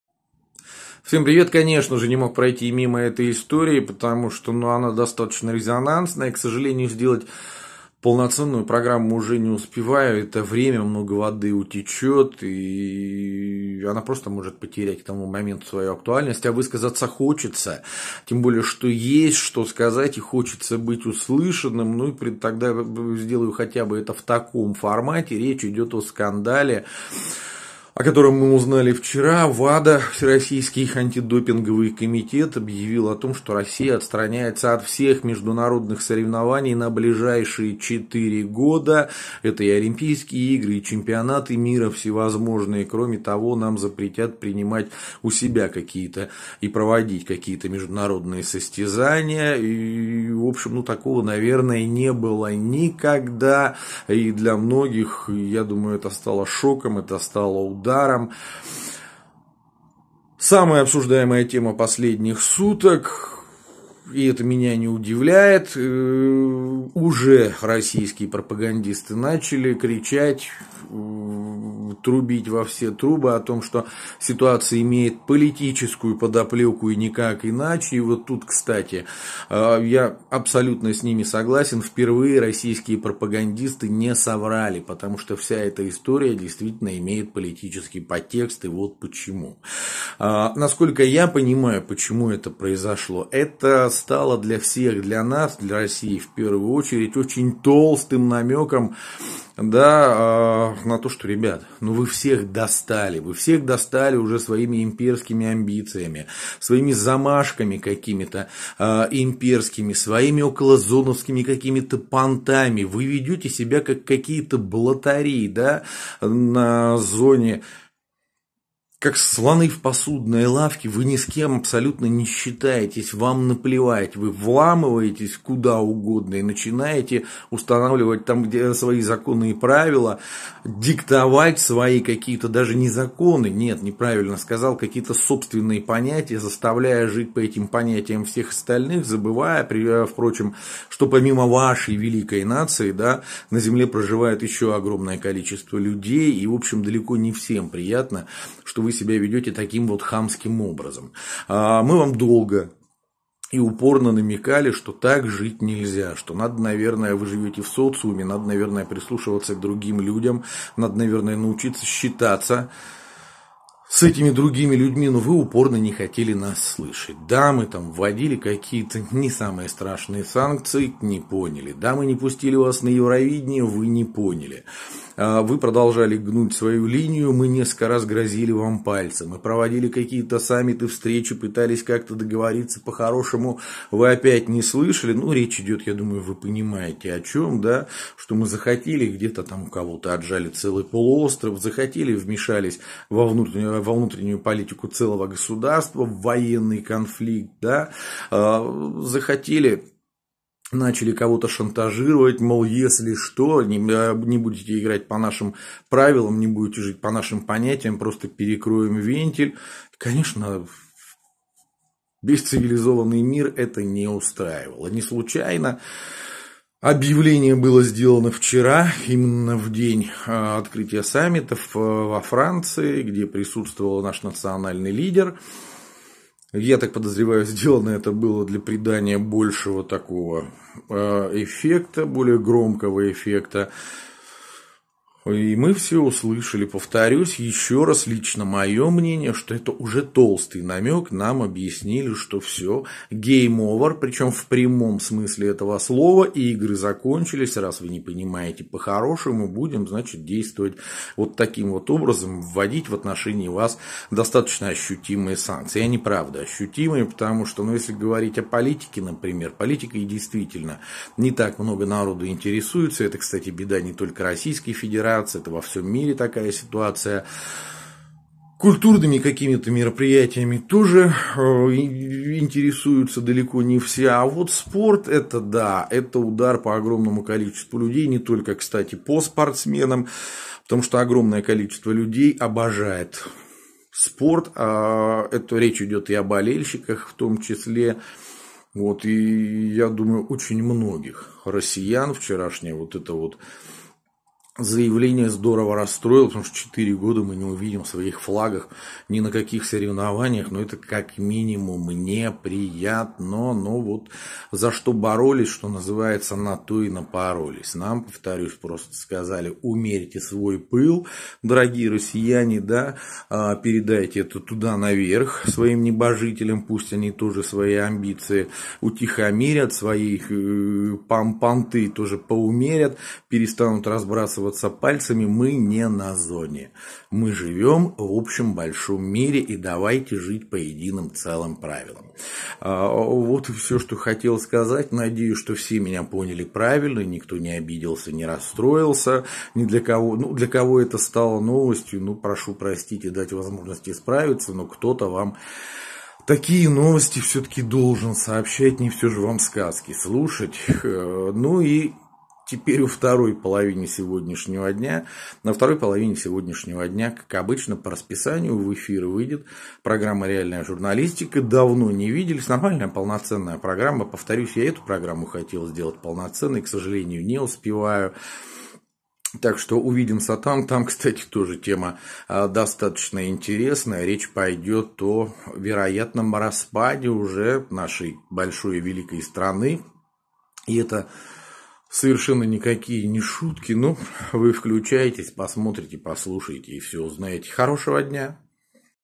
WADA: Гейм овер, дорогая Россия. Мир дал понять, что все устали от наших понтов и имперских амбиций
Четыре года без флага. WADA отстранил Россию от международных соревнований на четыре года. Конечно же это стало самой обсуждаемой темой последних суток, конечно же я не смог пройти мимо. И не смотря на то, что сделать полноценную программу по данному поводу не успеваю, хочу высказаться хотя в формате видеоблога. Безусловно это решение является политическим. Хотя бы в этот раз пропагандисты не соврут. Но, оно было и вполне прогнозируемым. Если мы упорно не хотим отключать борзометр и ведем себя так, как нам хочется (не считаясь с остальным миром).. оно просто обязано было случиться. Да, это очень толстый намек. Всем нам. И это действительно больно. Но.. за что боролись, что называется. Это "Реальная журналистика", рубрика "Коротко о главном", поехали!